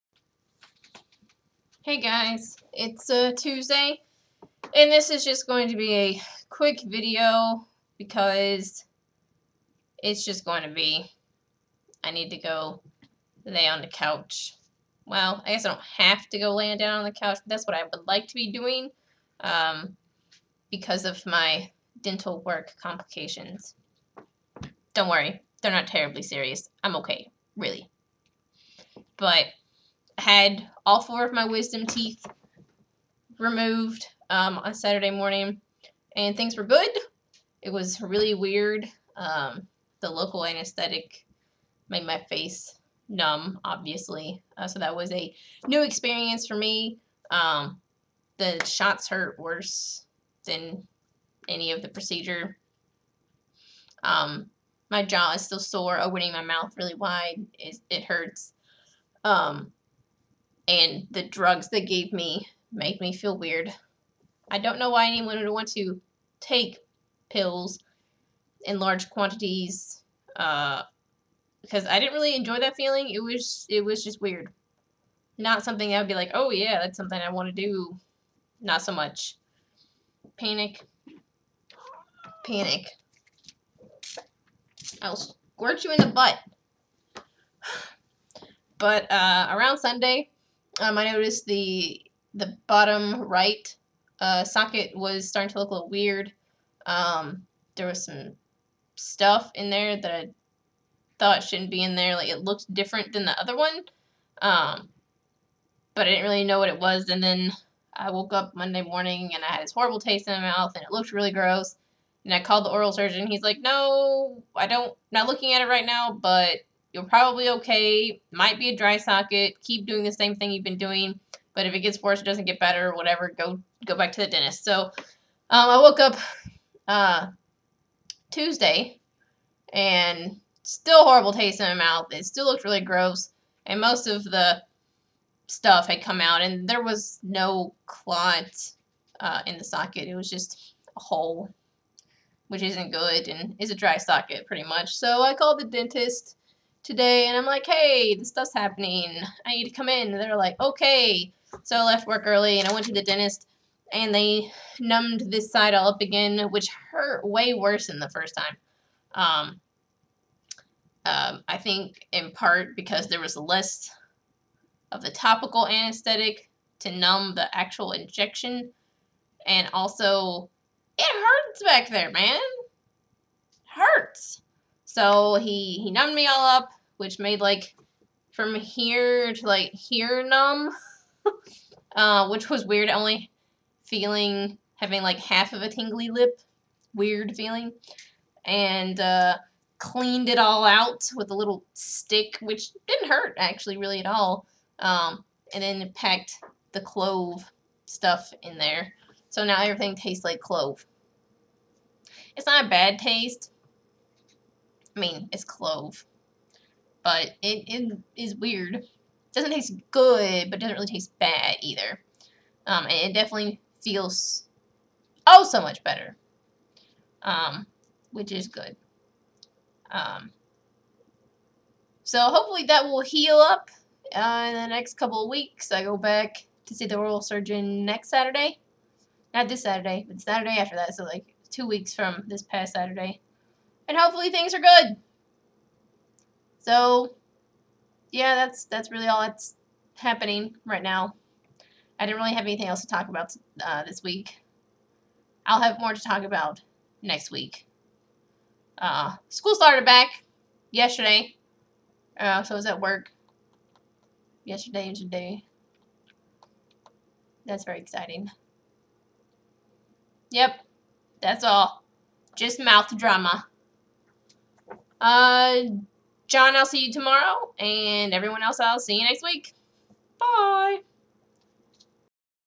one-take tuesday
I don't have much to talk about this week other than my missing teeth. Well...they aren't *missing* so much as they were removed from my mouth permanently. There was a complication, and I have one dry socket but that's ok. The clove stuff has stopped the pain, but makes things, including my mouth, taste weird. I am ok with this since it doesn't hurt anymore. My mashed potato and sherbet diet will have to continue for a few more days.